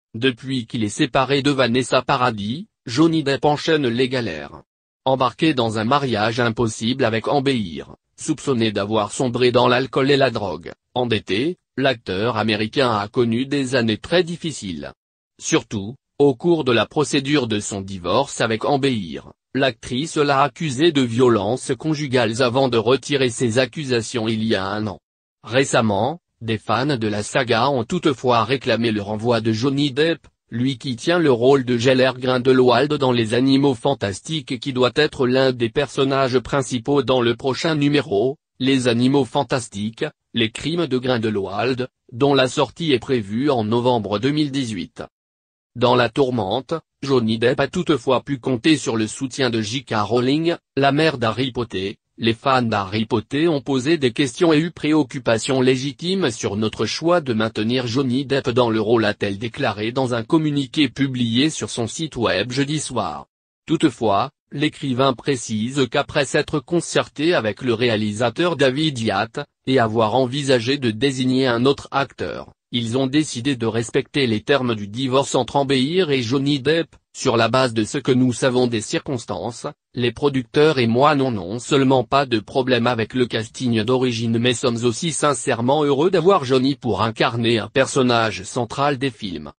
Johnny Depp accusé de violences conjugales : J.K. Rowling prend sa défense
Depuis qu’il est séparé de Vanessa Paradis, Johnny Depp enchaîne les galères. Embarqué dans un mariage impossible avec Amber Heard, soupçonné d’avoir sombré dans l’alcool et la drogue, endetté, l’acteur américain a connu des années très difficiles.